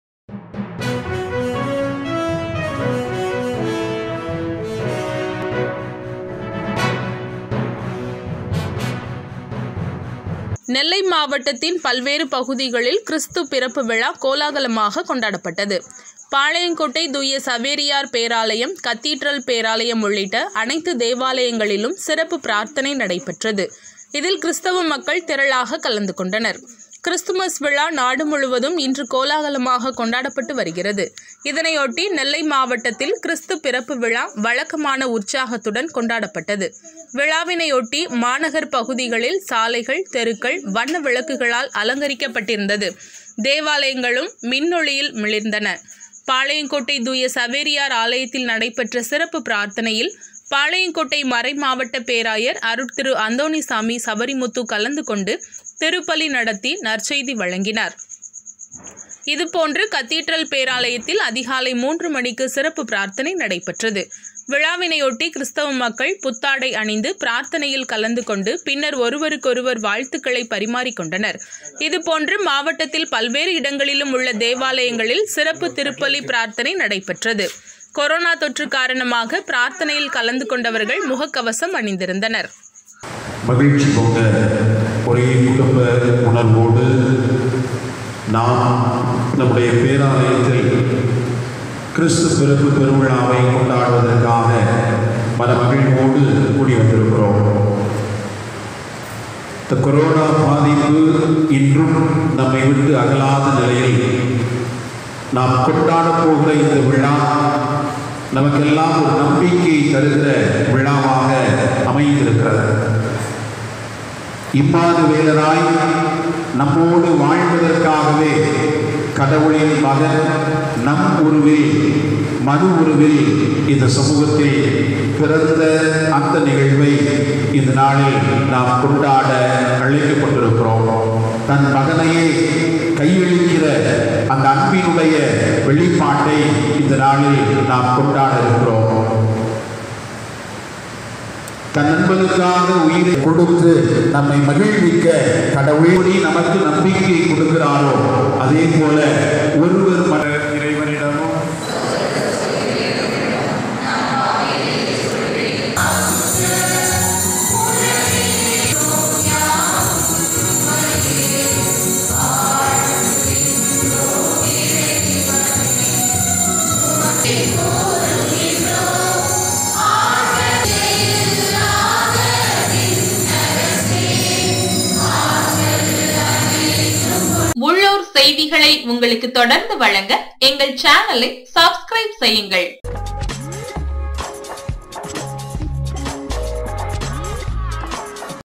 கிறிஸ்துமஸ் தினத்தை முன்னிட்டு சேவியர் கதீட்ரல்,தேவாலயத்தில் பிராத்தனைகள் நடைப்பெற்றது
M.கணபதி

நெல்லை மாவட்டத்தின் பல்வேறு பகுதிகளில் கிறிஸ்த்து பிறப்பு விழா கோலாகலமாக கொண்டாடப்பட்டது. பாளையங்கோட்டை தூய சவேரியார் பேராலயம், கத்தீட்ரல் பேராலயம் உள்ளிட்ட அனைத்து தேவாலயங்களிலும் சிறப்பு பிரார்த்தனை நடைபெற்றது. இதில் கிறிஸ்த்தவ மக்கள் திரளாக கலந்து கொண்டனர்.

  கிறிஸ்த்துமஸ் விழா நாடு முழுவதும் இன்று கோலாகலமாக கொண்டாடப்பட்டு வருகிறது. இதனையொட்டி நெல்லை மாவட்டத்தில்  கிறிஸ்த்து பிறப்பு விழா வழக்கமான உற்சாகத்துடன் கொண்டாடப்பட்டது. விழாவினையொட்டி மாநகர் பகுதிகளில் சாலைகள், தெருக்கள் வண்ண விளக்குகளால் அலங்கரிக்கப்பட்டிருந்தது. தேவாலயங்களும் மின்னொளியில் மிளிர்ந்தன. பாளையங்கோட்டை தூய சவேரியார் ஆலயத்தில்  நடைபெற்ற சிறப்பு பிரார்த்தினையில் பாளையங்கோட்டை மறைமாவட்ட பேராயர் அருட்திரு.அந்தோனி சாமி சவரிமுத்து கலந்து கொண்டு திருப்பலி நடத்தி நற்செய்தி வழங்கினார். இதுபோன்று கத்தீட்ரல் பேராலயத்தில் அதிகாலை 3 மணிக்கு  சிறப்பு பிரார்த்தனை நடைபெற்றது. விழாவினையொட்டி கிறிஸ்தவ மக்கள் புத்தாடை அணிந்து பிரார்த்தனையில் கலந்து கொண்டு பின்னர் ஒருவருக்கு ஒருவர் வாழ்த்துக்களை பறிமாறிக்கொண்டனர்.  இதுபோன்று மாவட்டத்தில்  பல்வேறு இடங்களிலும் உள்ள தேவாலயங்களில் சிறப்பு திருப்பலி பிரார்த்தனை நடைபெற்றது . கொரோனா தொற்று காரணமாக பிரார்த்தனையில் கலந்து கொண்டவர்கள் முக கவசம் அணிந்திருந்தனர்

பேட்டி: அந்தோனி சாமி சவரிமுத்து (பாளை மறைமாவட்ட ஆயர்)